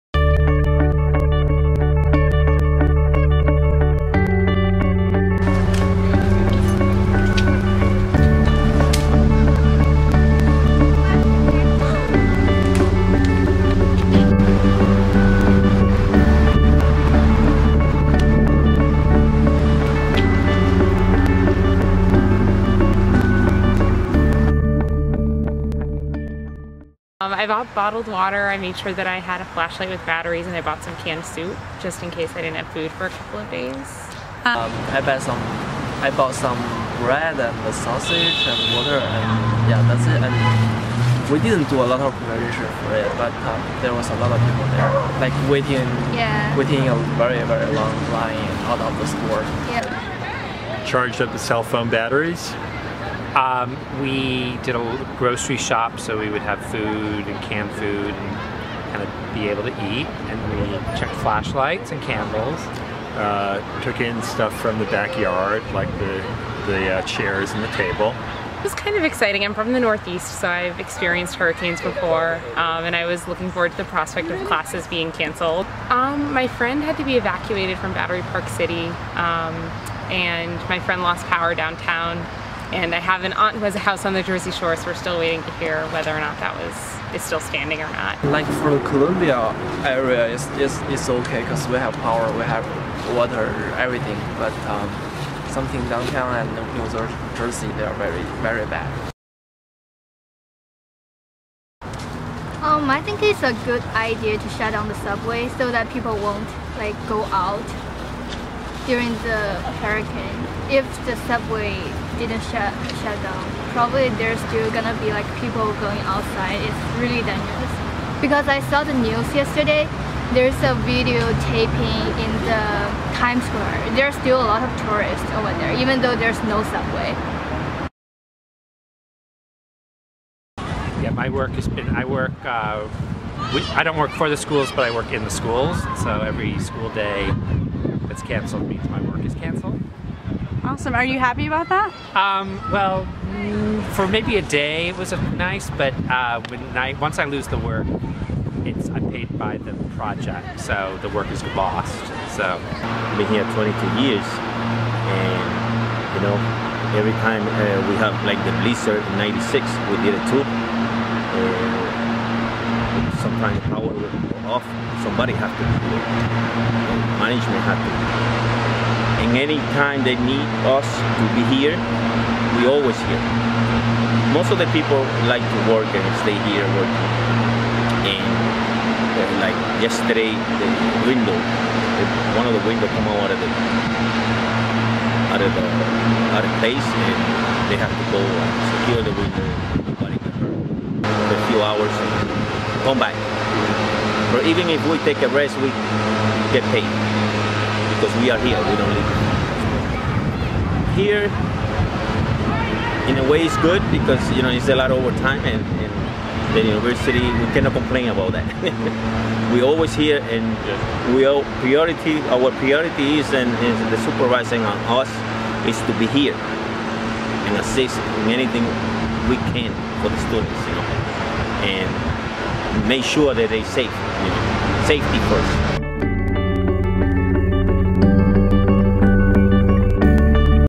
Hurricane Aftermath in Morningside Heights
Morningside Heights residents and students respond to the hurricane aftermath.
Filmed and edited by Nino Rekhviashvili.